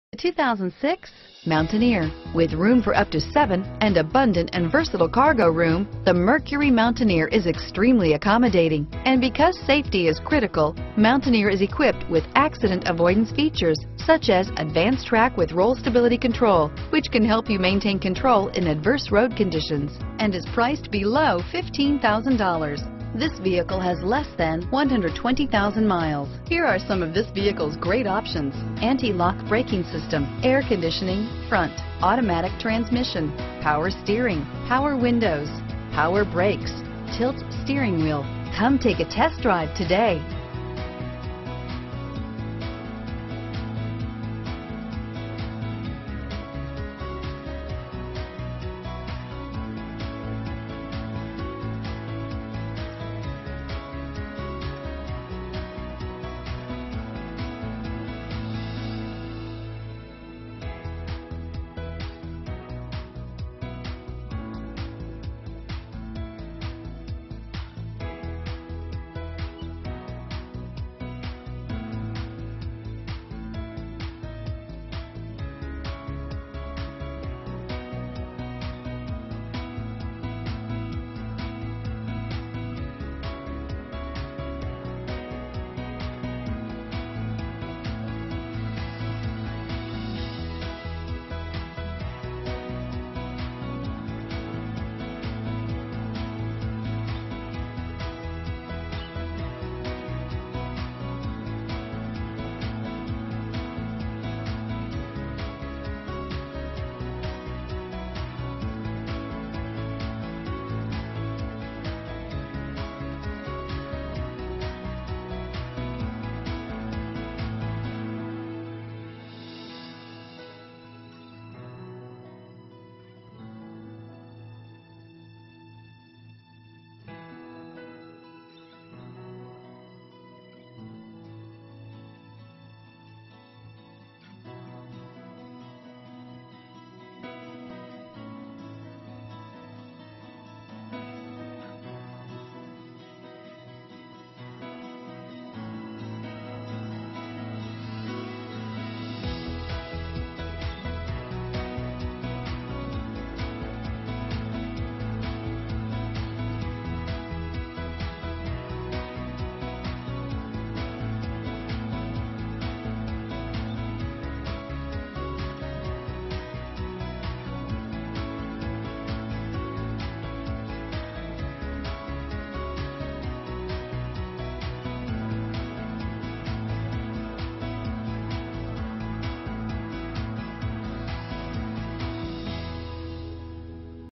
2006 Mercury Mountaineer Reno, Carson City, Northern Nevada, Elko, Fallon, NV UT1147A
2006 Mercury Mountaineer
Nevada.
2006 Mercury Mountaineer Premier - Stock#: UT1147A - VIN#: 4M2EU48846UJ15210

Excellent ride and handling characteristics for a truck-based SUV, strong V8, comfortable cabin with user-friendly layout, standard Roll Stability Control. Unbeatable Wild West Pricing!